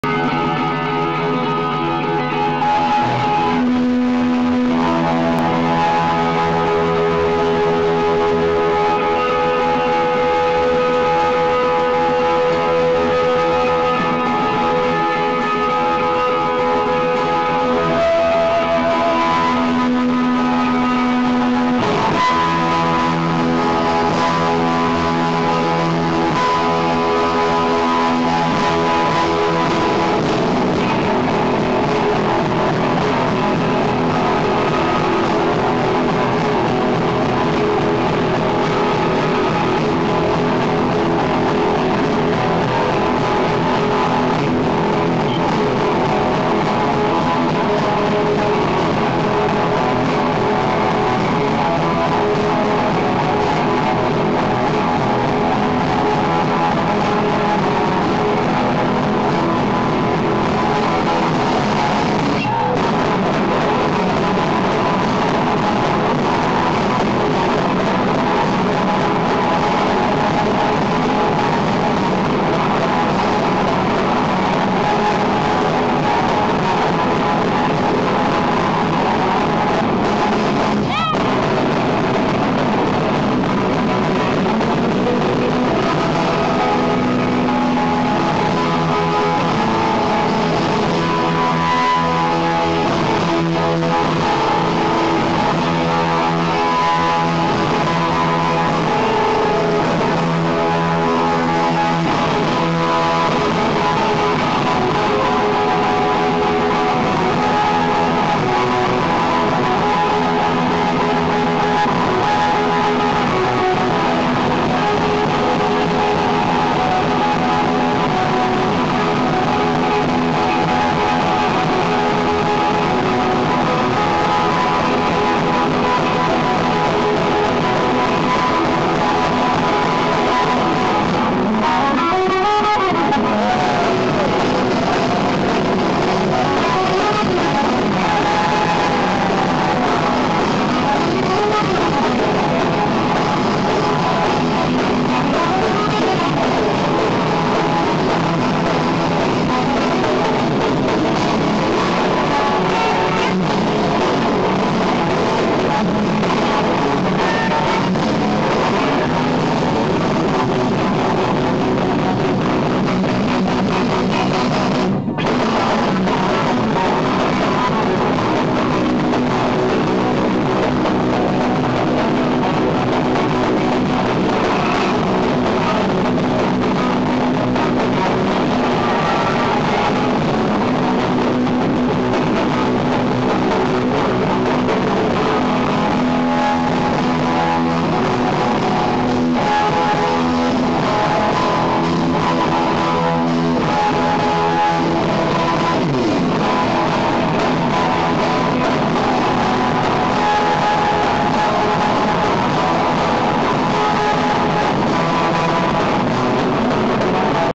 Pneu à l'after du AAA Fest @ Grnd Zéro Gerland
Live de Pneu @ grnd zéro gerland in Lyon à l'after du AAA Fest